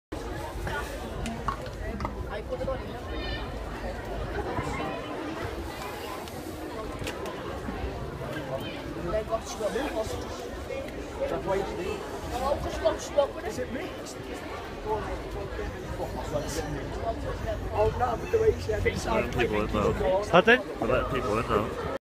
O2 Academy Liverpool queue
Queuing up to see Eminem